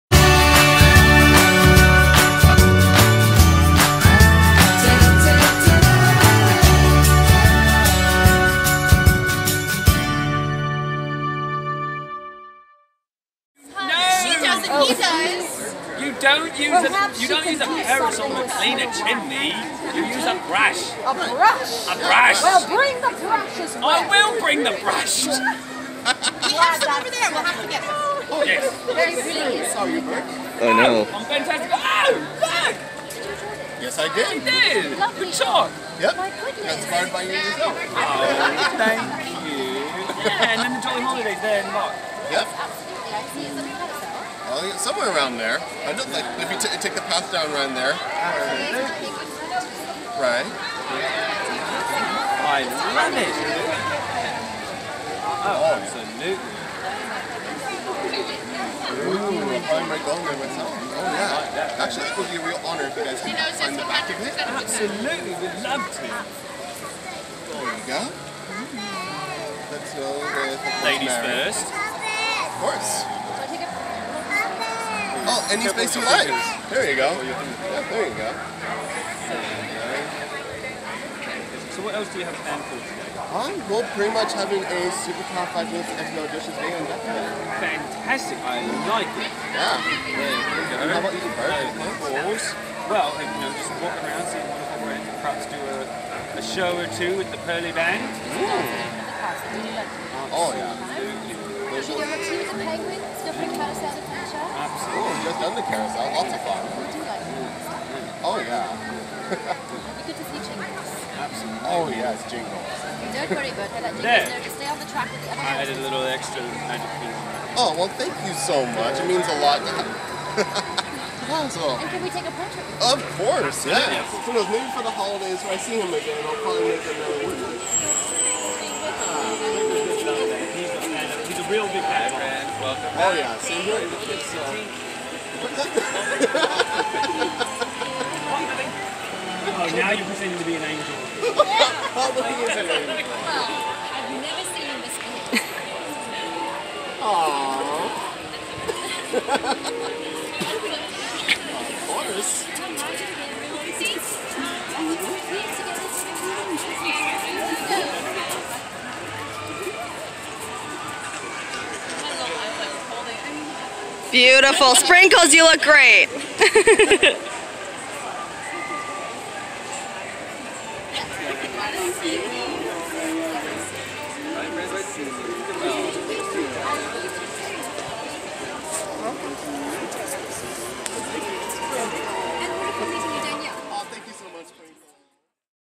Practically Perfect Fun with Mary Poppins, Bert and Sprinkles the Penguin
Another Character Interaction video is here! Join Jake as he meets Mary Poppins, Bert and Sprinkles the Penguin on a practically perfect day at Disneyland. Here, Jake has brought a chalk drawing that he has done of Bert's park drawing from the film for them to sign. During the second visit, Sprinkles the penguin makes a surprise appearance.